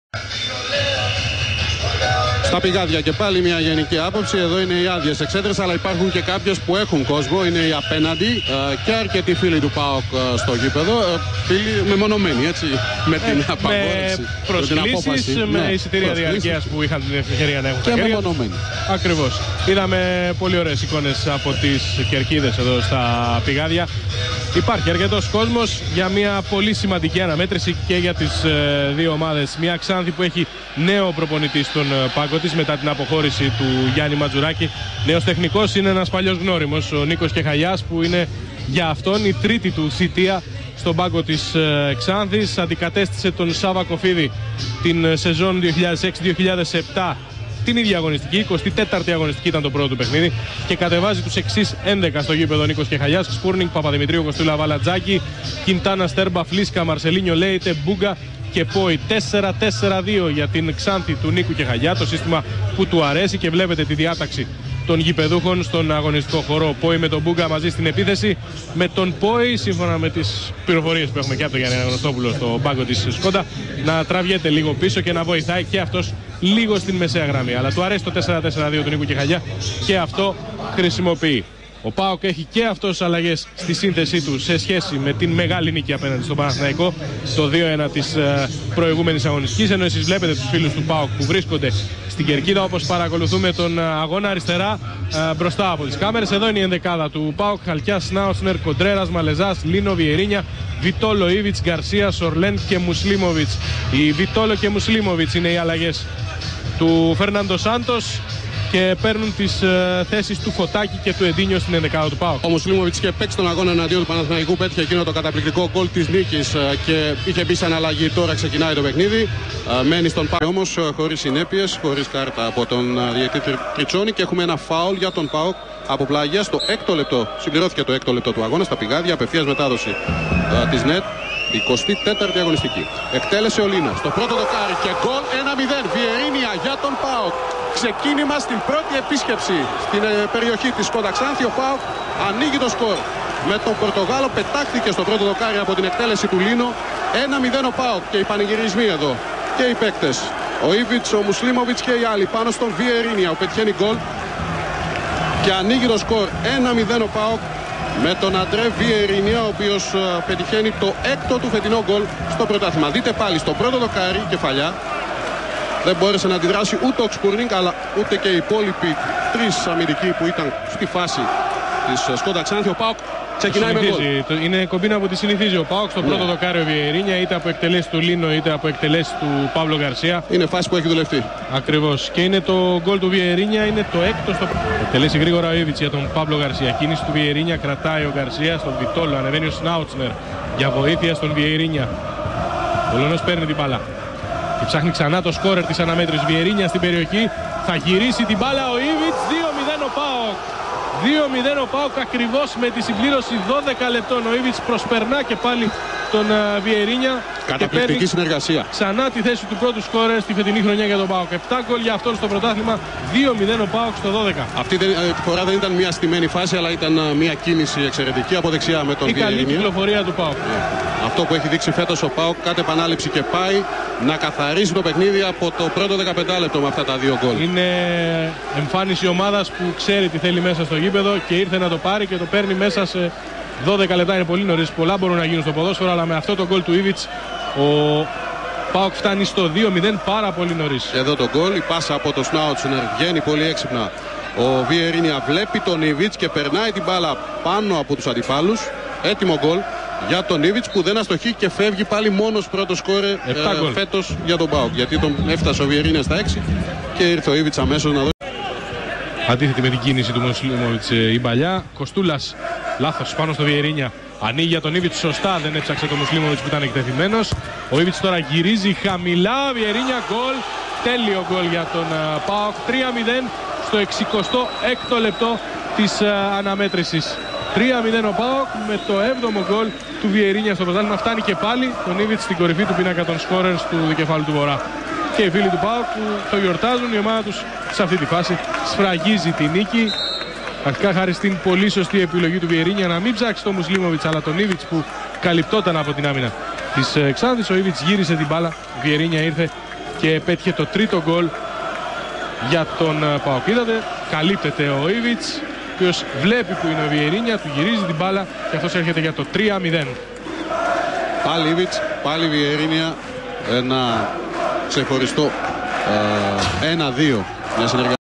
ΞΑΝΘΗ - ΠΑΟΚ 0-3 2009-10
Ο "δικέφαλος" πήγε με έναν και μοναδικό στόχο στην Ξάνθη και αυτός ήταν δεν ήταν άλλος από τη νίκη. Η ομάδα του Φερνάντο Σάντος έδειξε από νωρίς τις διαθέσεις της και μόλις στο 7ο λεπτό προηγήθηκε με κεφαλιά του Βιεϊρίνια. Έξι λεπτά αργότερα ο Ίβιτς, με νέα κεφαλιά, έδωσε στον ΠΑΟΚ αέρα δύο τερμάτων, ενώ στο 66ο λεπτό ο Βιεϊρίνια και πάλι διαμόρφωσε το τελικό σκορ.

άνθη (Κεχαγιάς): Γκσπούρνινγκ, Παπαδημητρίου (46' Στίνγκα), Φλίσκας, Κωστούλας, Τζάγκι, Μπούγκα (46' Έλινγκτον), Μαρσελίνιο, Στρμπα, Πόι, Βάλλας, Κιντάνα (68' Λουτσιάνο)

ΠΑΟΚ (Σάντος): Χαλκιάς, Γκαρσία (89' Εντίνιο), Βιτόλο, Σορλέν, Μουσλίμοβιτς, Μαλεζάς, Κοντρέρας, Λίνο, Βιεϊρίνια (79' Κριστιάνο), Ίβιτς (85' Ίβιτς), Σνάουτσνερ

Κίτρινες κάρτες: Λουτσιάνο

Διαιτητής: Σ. Τριτσώνης (Αθηνών)